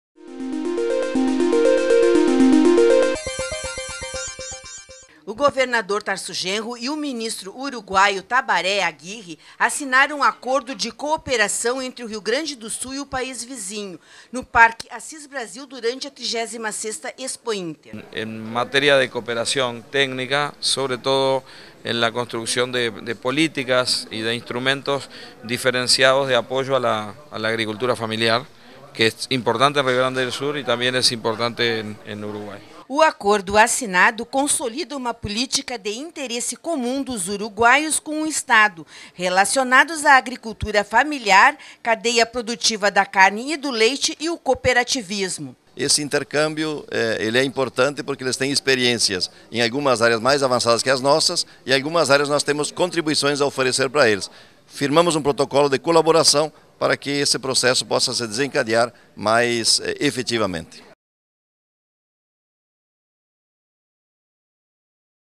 RS e Uruguai firmam parceria para promoção do desenvolvimento rural
O fortalecimento das políticas públicas de assistência técnica, institucional, organização social, cooperativismo e implementação de programas em agricultura e aquicultura familiar e da pecuária são objetivos de um termo de cooperação firmado entre o Estado e o Ministério da Agricultura, Pecuária e Pesca do Uruguai.